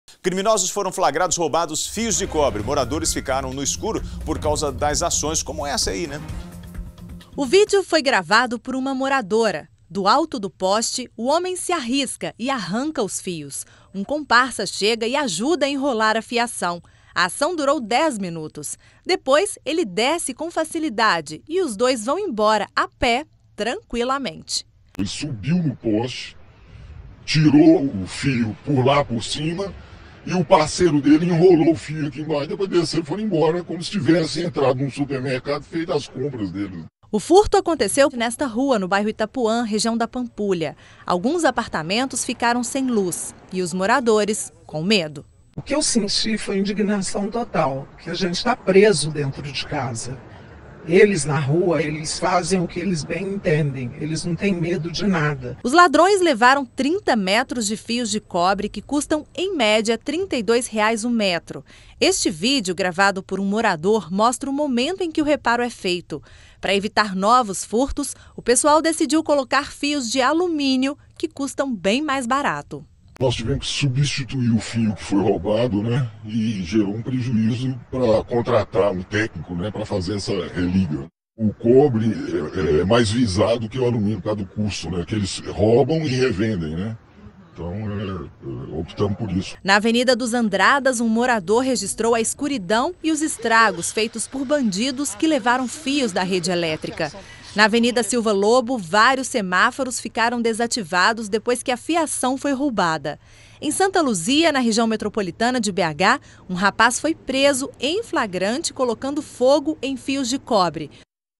Moradora flagra bandidos furtando fios de cobre em poste
Criminosos foram flagrados roubando fios de cobre. Moradores ficaram no escuro por conta da ação dos bandidos. Uma moradora conseguiu gravar o momento em que os fios foram roubados.